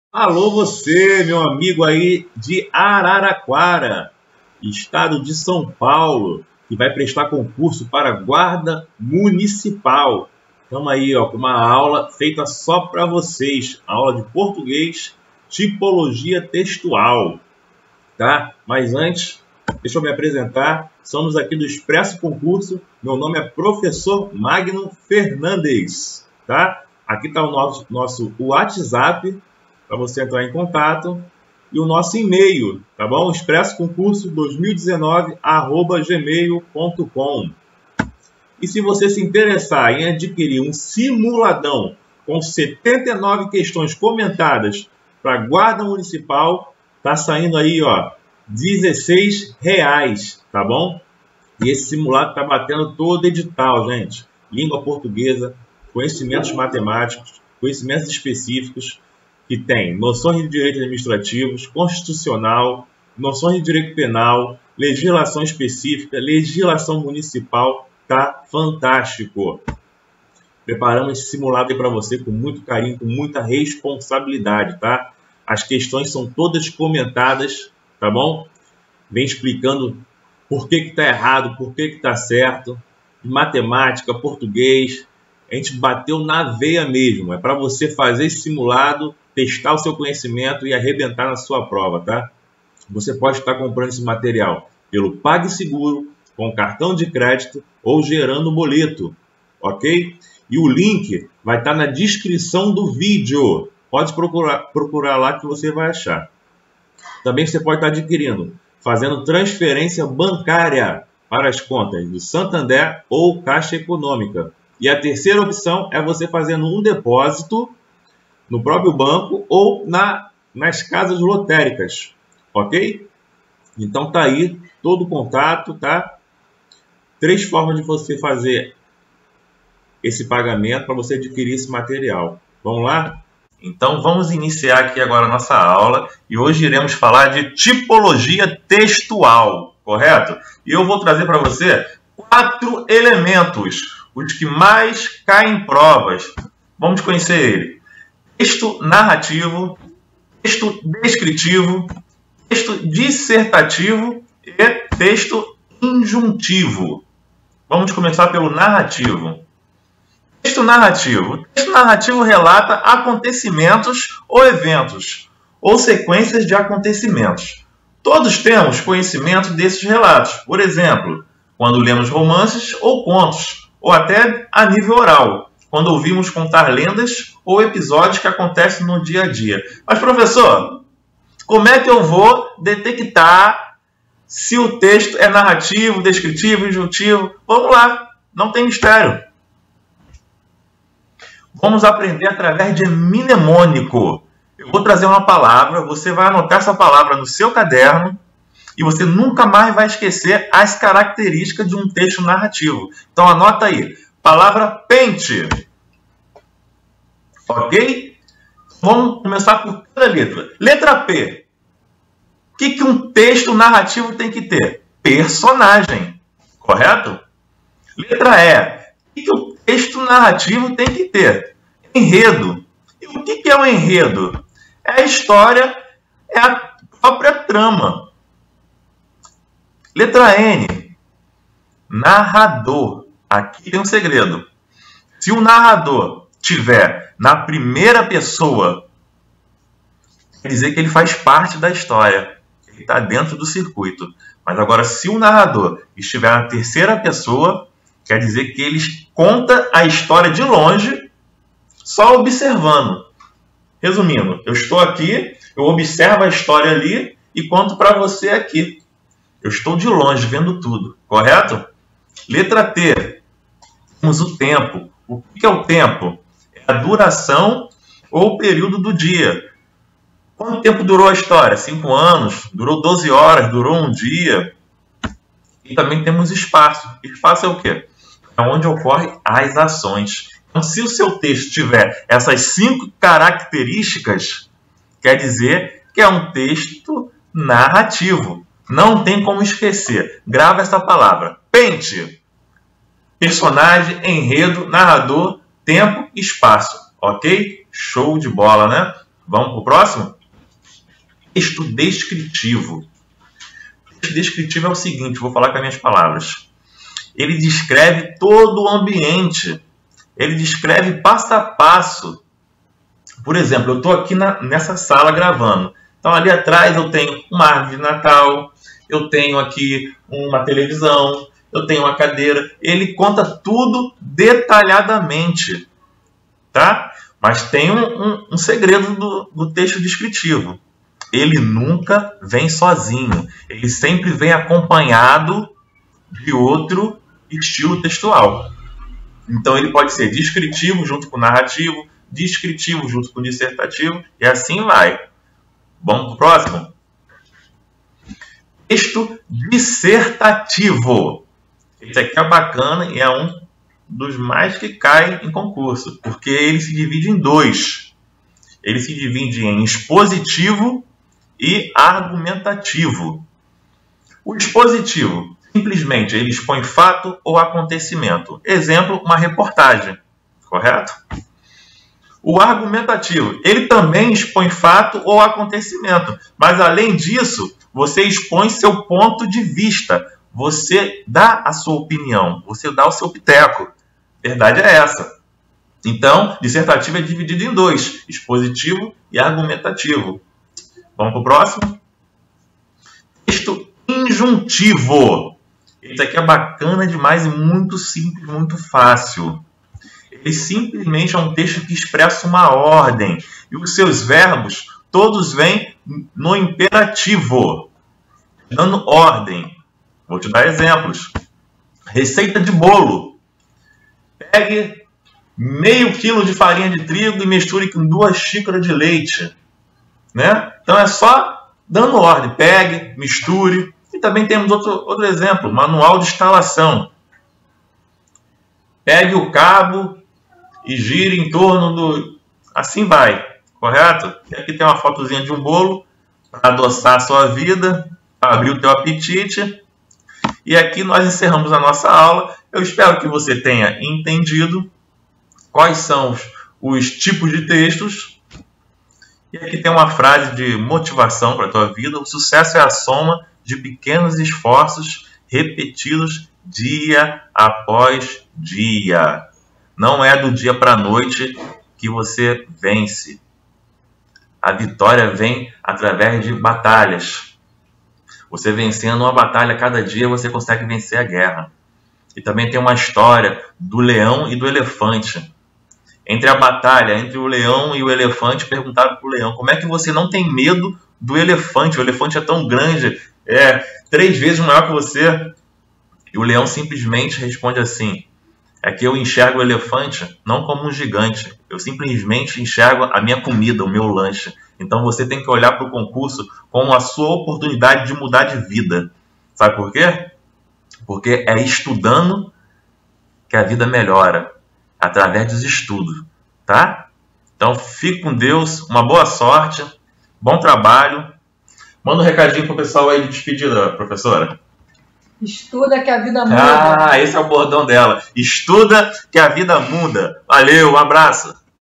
AULA GUARDA MUNICIPAL ARARAQUARA SP - SIMULADÃO COMENTADO 2020.
GUARDA MUNICIPAL ARARAQUARA SP
AULA DE PORTUGUÊS - TIPOS TEXTUAIS
SIMULADO COMPLETO BATENDO TODO EDITAL